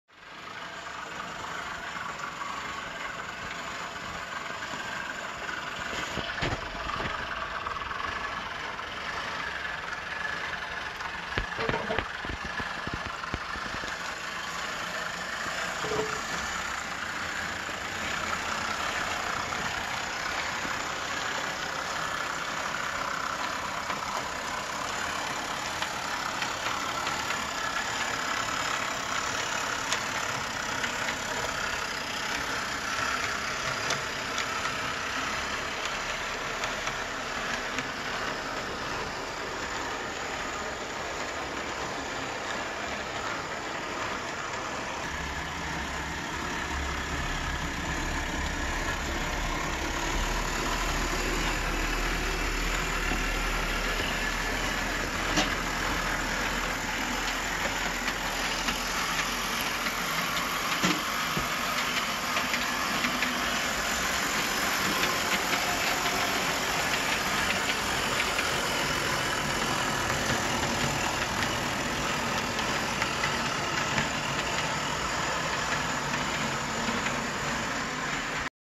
Burlington steam engine mallet T2 vintage O scale built by Bernard G Corbin
Burlington steam engine mallet T2 vintage O scale built by Bernard G Corbin pulling cars also made by Corbin. Beautifully scratch built piece ..Corbin was a master builder 1930's -1970's